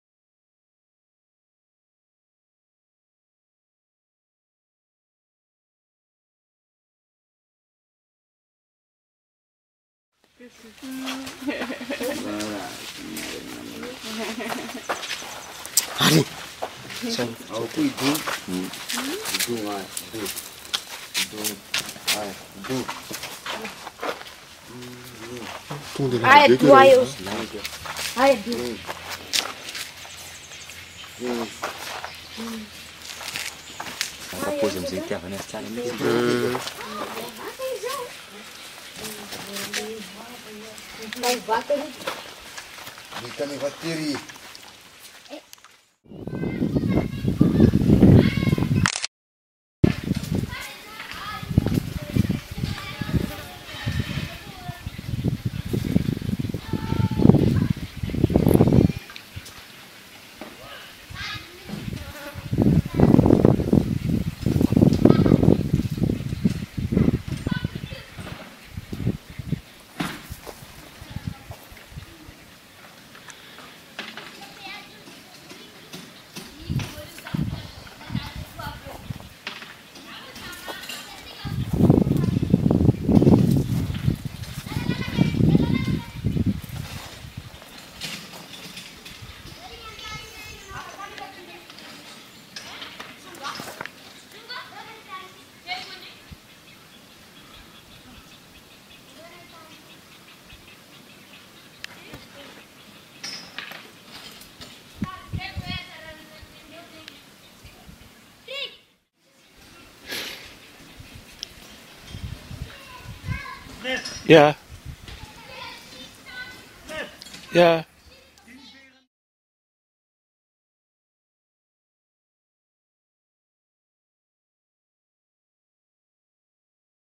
Snakes in the garden of Zazamalala (Antsirabao, Madagascar)
This video shows two different endemic snake species in the garden van Zazamalala (Antsirabao, near Morondava, Madagascar)